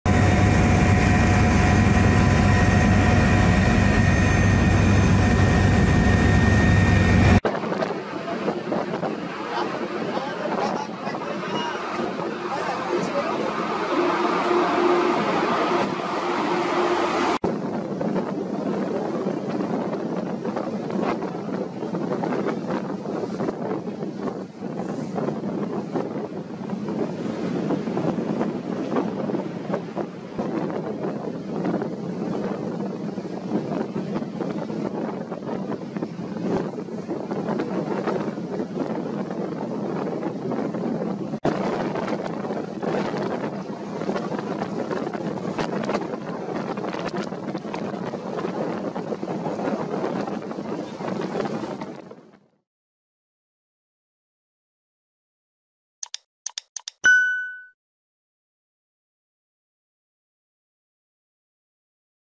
PH tanker sinks, causes massive oil spill off Bataan
A Philippine-flagged tanker carrying 1.4 million liters of industrial fuel oil capsized and sank off Manila on Thursday, authorities said, as they seek to contain a spill stretching several kilometres.

AFP VIDEO